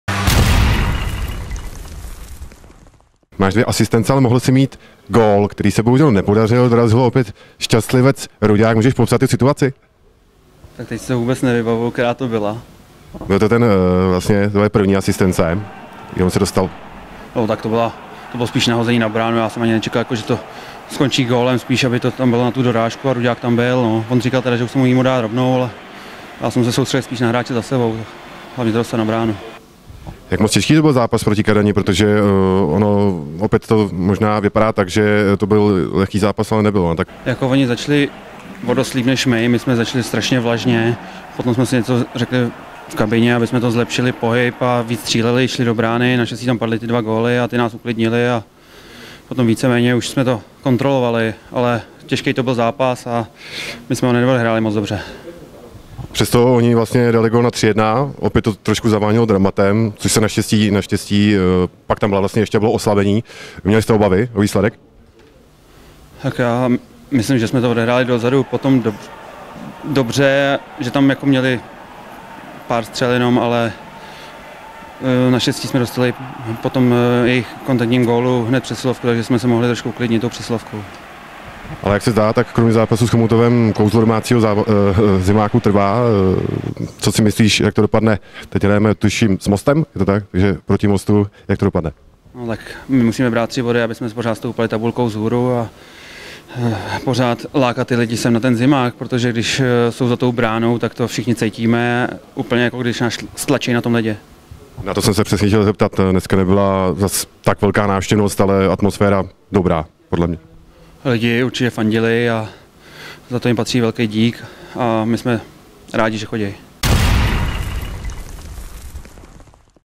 David Růžička: Dneska bylo nutné chodit hodně do brány
Výrazný bek Rytířů byl znovu vidět. Stál v pozadí dvou gólů přesto nebyl s výkonem týmu proti Kadani úplně spokojen. Potěšila ho naopak divácká podpora, která je pořád skvělá.